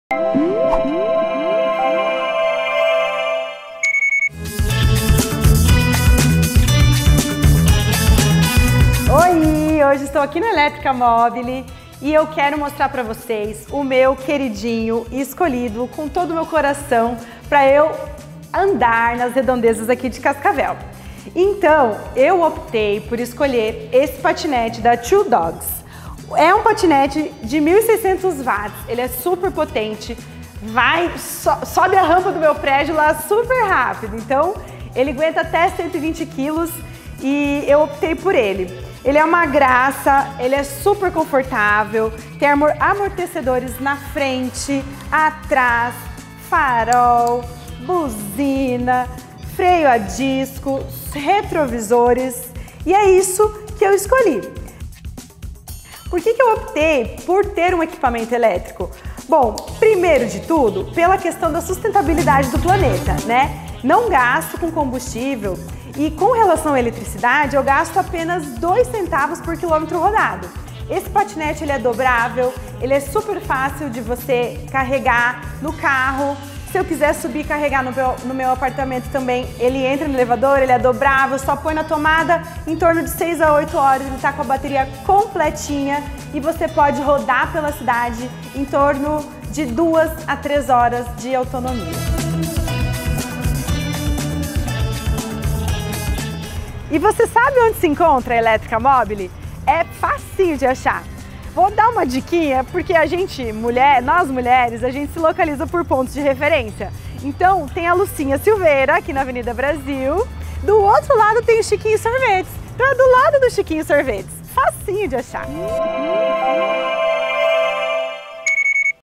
Vá trabalha de Patinete é ganhe tempo no seu dia!!
Você já pensou na possibilidade de se libertar do carro e do estresse por conta do trânsito?